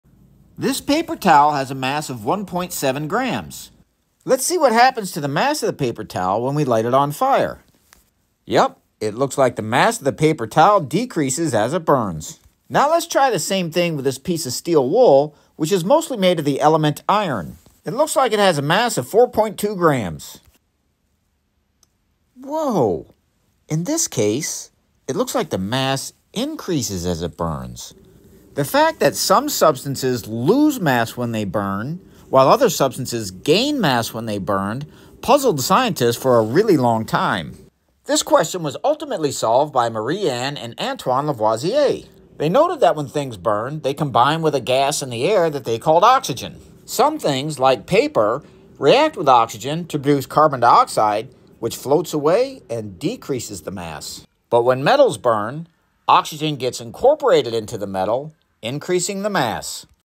Paper vs. metal
Why do metals burn differently than carbon-containing materials?